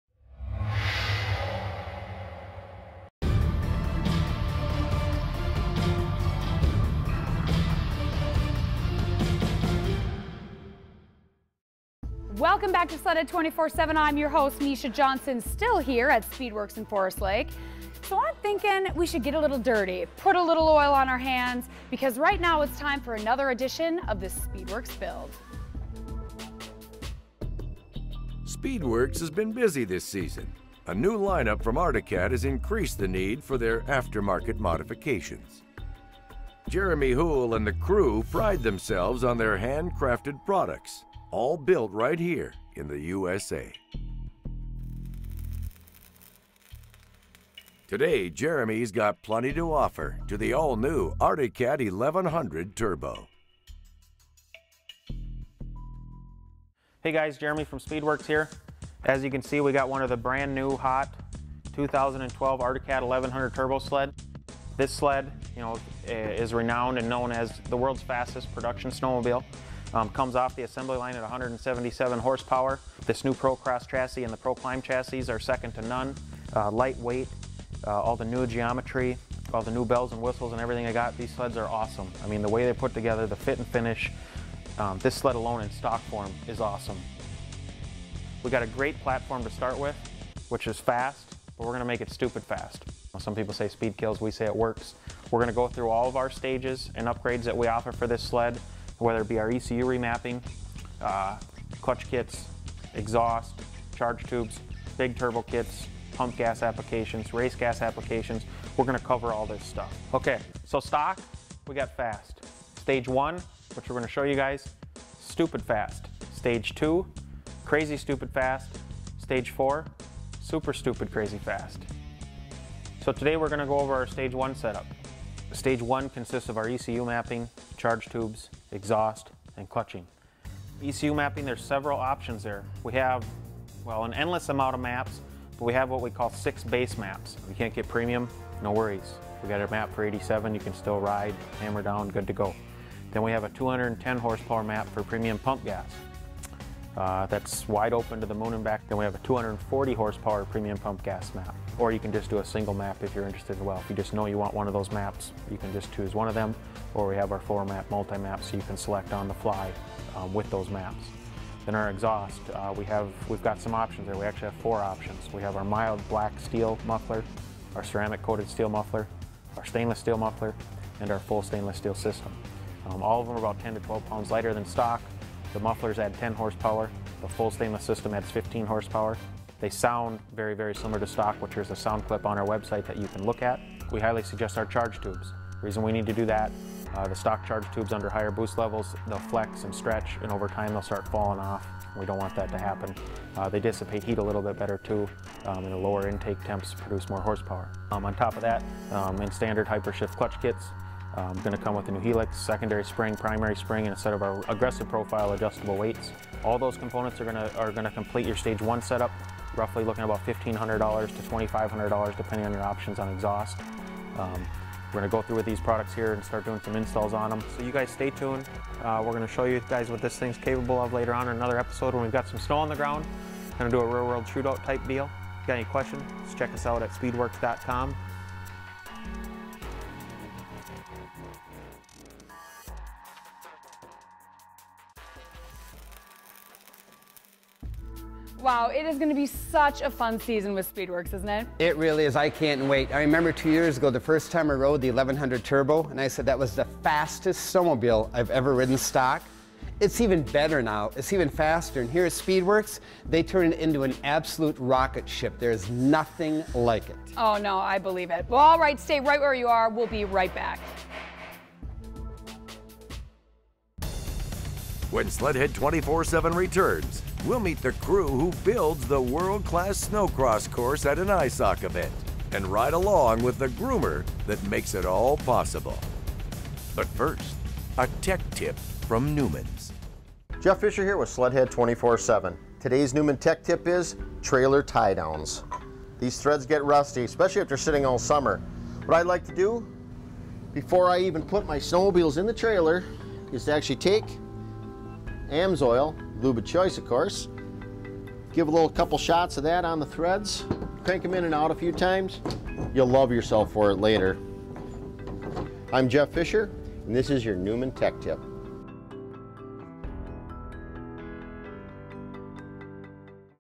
Show 5 Act 3 of 4 Speedwerx F1100 Turbo Mods
Speedwerx is capable of making the fastest snowmobile in the world.  Check out what Jeremy has for the fastest sled on snow, the 2012 Arctic Cat F1100 Turbo...